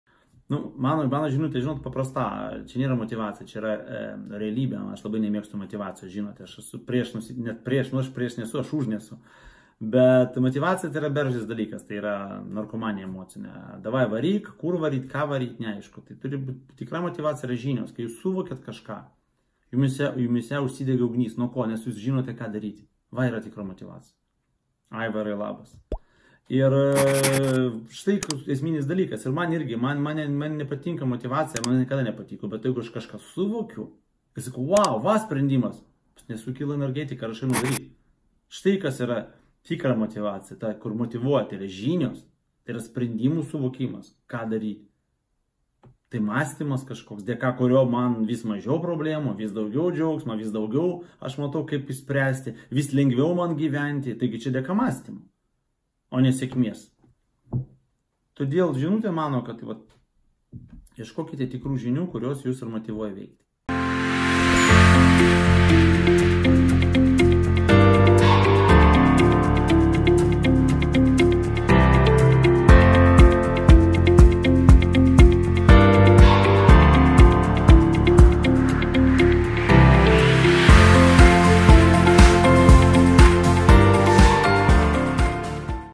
Kodėl netikiu "motyvacija"? Kas man yra TIKRA motyvacija. Jevgenijus Černyš
Ne kartą domėjotės ir galvojote kaip rasti daugiau motyvacijos. Žiūrėjote vaizdo įrašus, galbūt lankėtės motyvaciniuose seminaruose ar pan. Mano nuomone visa tai yra apsimetinėjimas. Tikra motyvacija yra visiškai kitas dalykas. Apie tai šiame vaizdo įraše.

Pradėk nuo idėjos. O tada verslo.